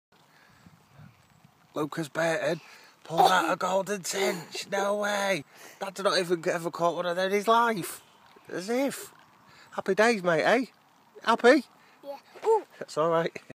Lucas catches a very rare golden tench
fishing tench